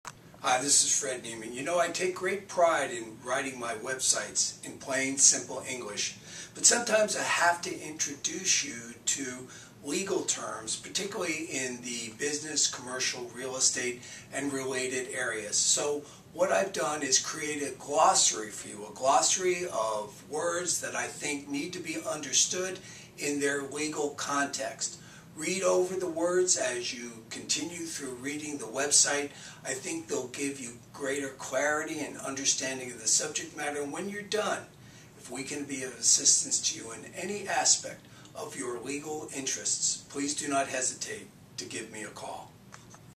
Business & Corporate Glossary
Vocabulary words have meaning both in life and especially in the business world particularly as the business world must work under the rules and the laws of the state and states in which it does business.  Fredrick P. Niemann, Esq. of Hanlon Niemann & Wright, freehold Township, Monmouth County, NJ, business and corporate law attorney identifies those frequently used words used in this website and what the words mean in the context of the business community.  The words are listed in alphabetical order and written precise and generally simple English.  Mr. Niemann invites his viewers and readers to contact him should they require further assistance in their New Jersey business and/or corporate enterprise.